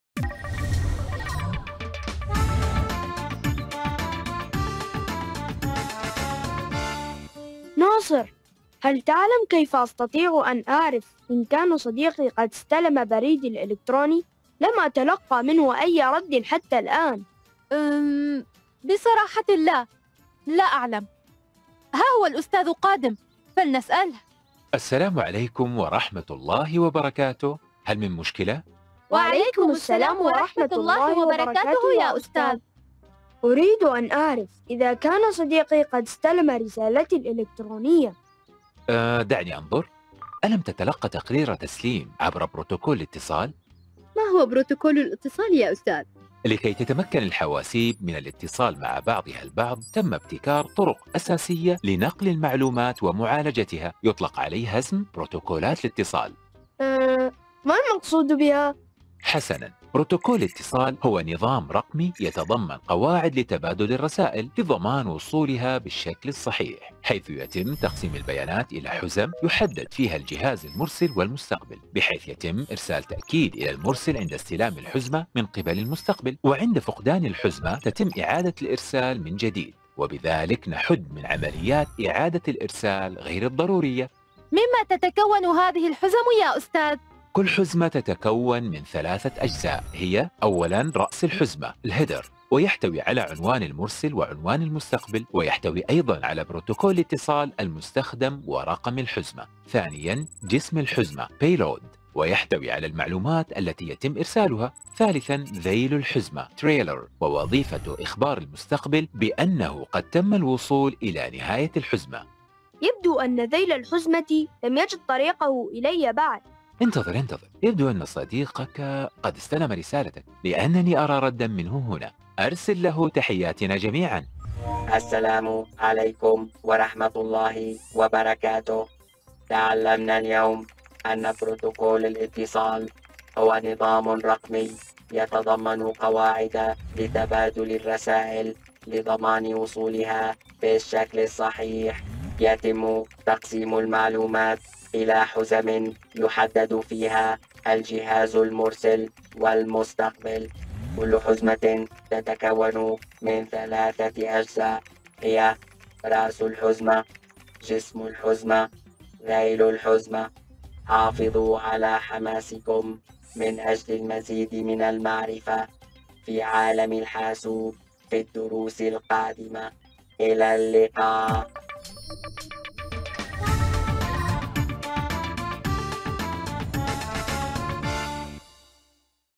بروتوكولات الاتِّصال في الشَّبَكَات | Network communication protocols
"يَتحدَّث هذا المحتوى المَرْئِيّ عن بروتوكول الاتِّصال في الشَّبَكَات وهو نظامٌ رقميّ يتضمَّن
قواعد لتبادُل الرَّسائل؛ لضمان وصولها بالشَّكل الصحيح؛ حيث يَتِمّ تقسيم المعلومات إلى حُزَم
يُحدّد فيها الجهاز المُرسِل والمُستقبِل؛ بحيث يَتِمّ إرسال تأكيد إلى المرسِل عند استلام الحُزْمة
مِن قِبَل المستقبِل. وعند فقدان الحزمة تتم إعادة الإرسال من جديد"
"This visual content deals with the communication protocol in networks, which is a digital
system that includes rules for exchanging messages. to ensure that it arrives properly;
Where the information is divided into packages in which the transmitter and receiver are
identified so that a confirmation is sent to the sender when the packet is received by the
receiver. When the packet is lost, it is retransmitted again."